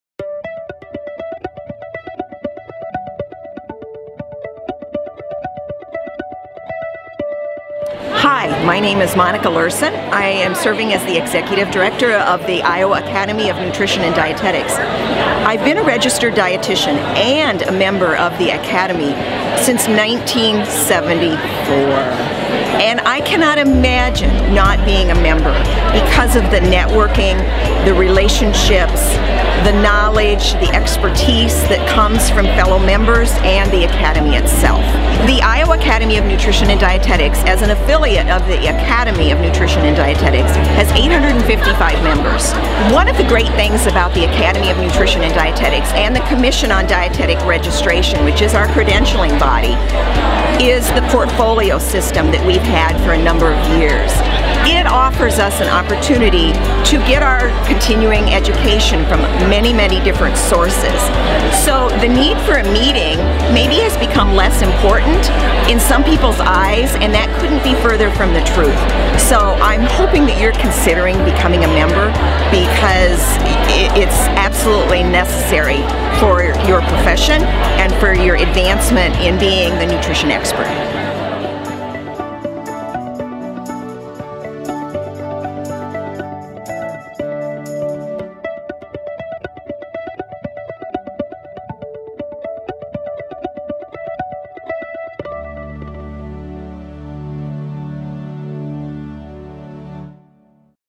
Monica Lursen - Iowa Academy of Nutrition and Dietetics
Iowa Academy of Nutrition and Dietetics
Benefits of Attending the "Annual Meeting"

Registered Dietitians Are The Real Nutrition Experts
Contact One TODAY!